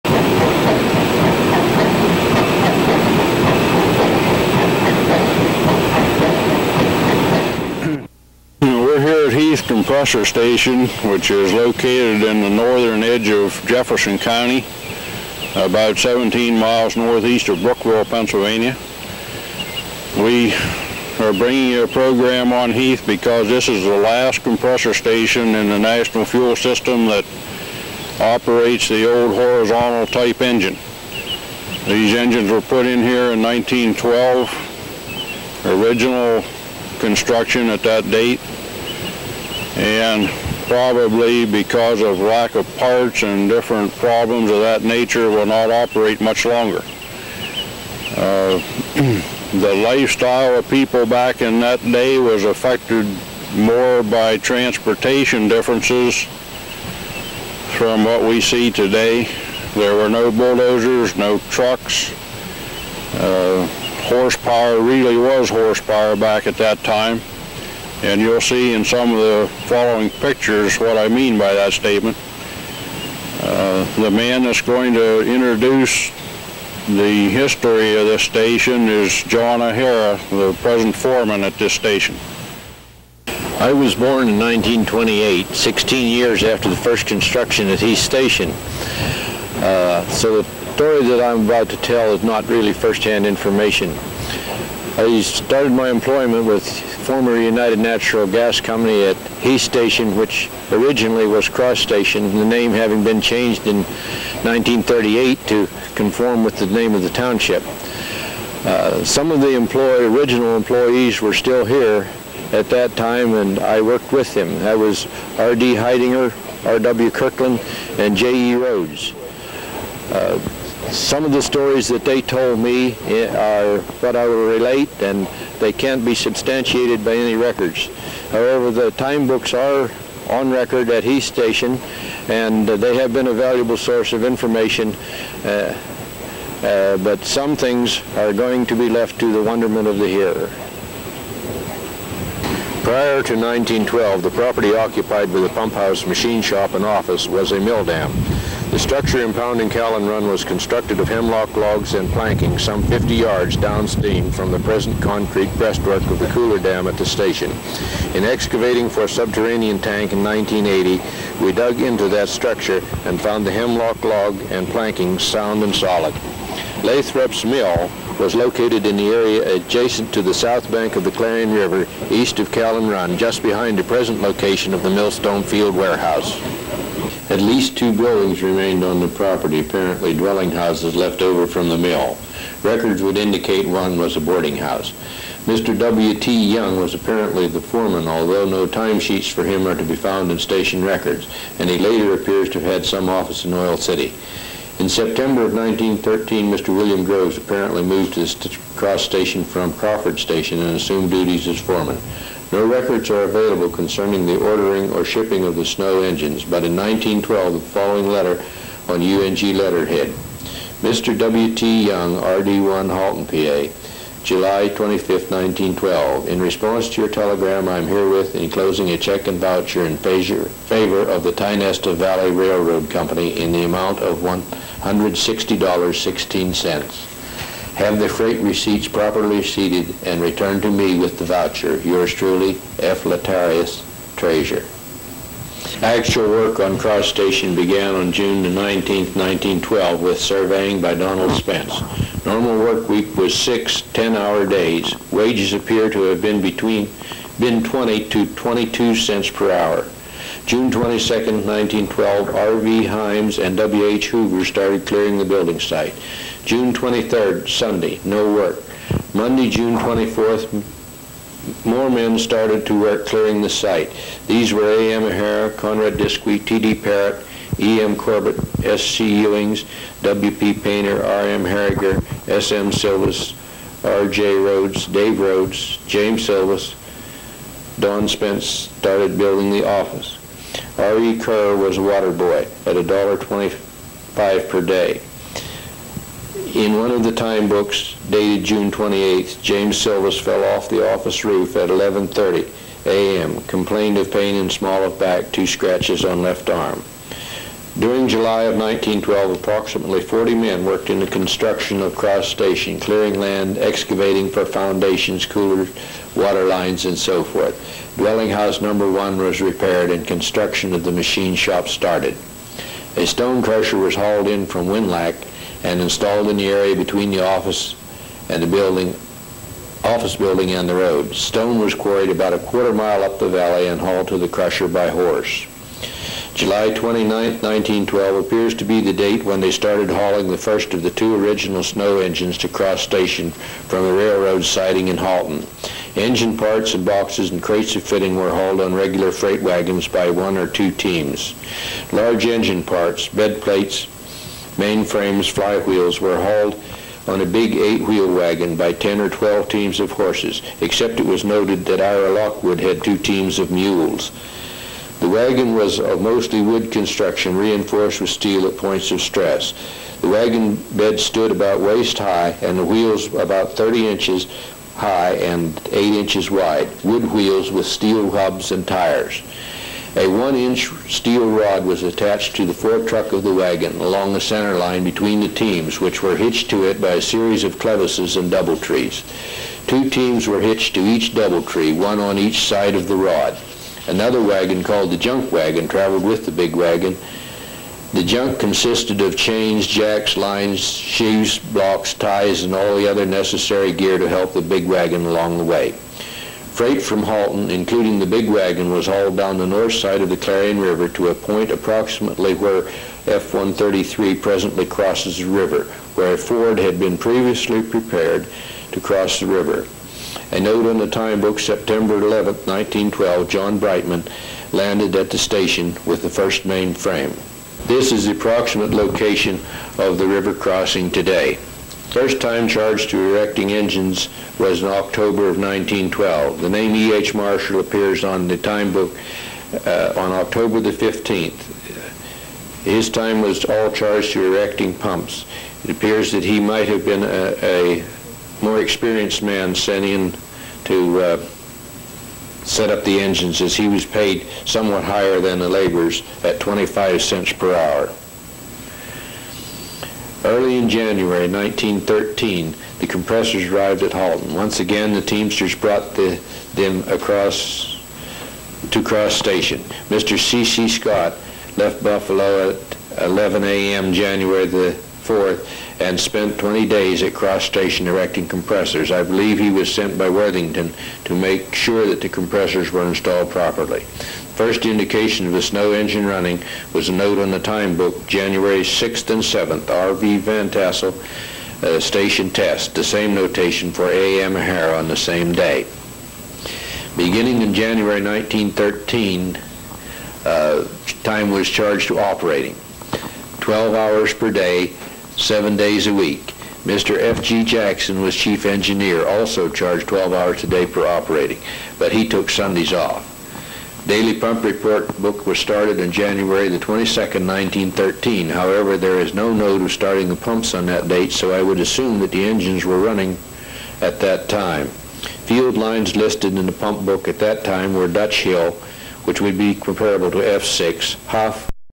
Heath Pumping Station Documentary Part1
Part 1 Documentary by National Fuel  on the Heath , Pa Pumping Station.  there are (6) 450 HP Snow Steam Engine Co natural gas pumping engines.  Video taken in 1985